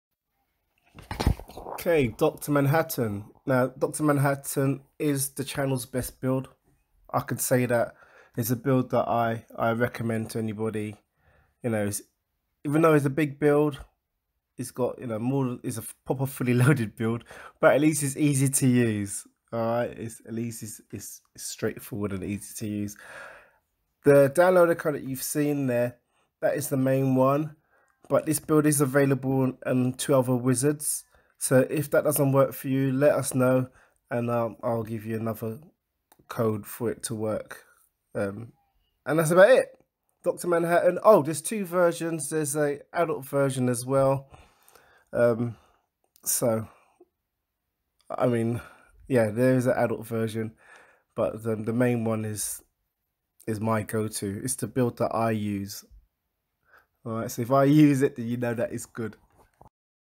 What is the Downloader Code for the Dr Manhattan Kodi Build
Install Kodi via the downloader code

You may need to relaunch the build a few times.

Zippo’s Media
Forks 867976

best addons